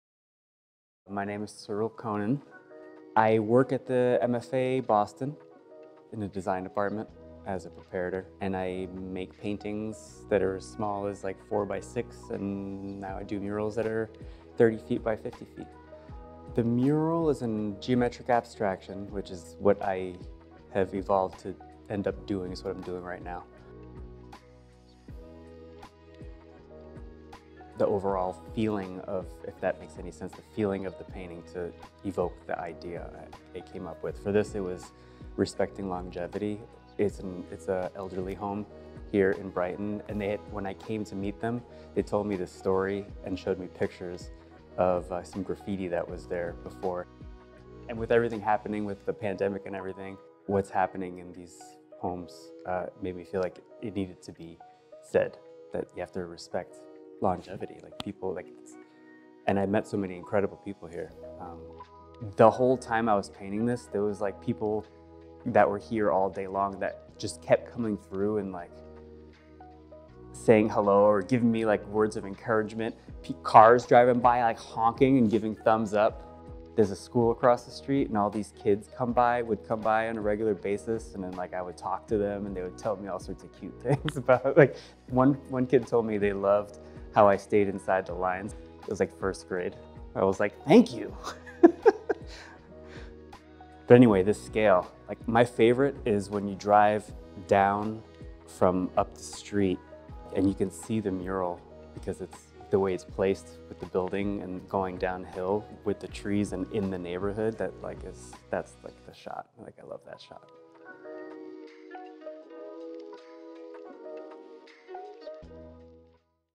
Transformative Public Art Program - Cyrille Conan
Artist Cyrille Conan was commissioned to create a mural as part of Arts in Boston's Transformative Public Art Program. His finished product is "Doujans D’al Hirhoal (Respecting Longevity)" at the Patricia White Apartments in Brighton.

Learn more about the Transformative Public Art program at boston.gov/public-art.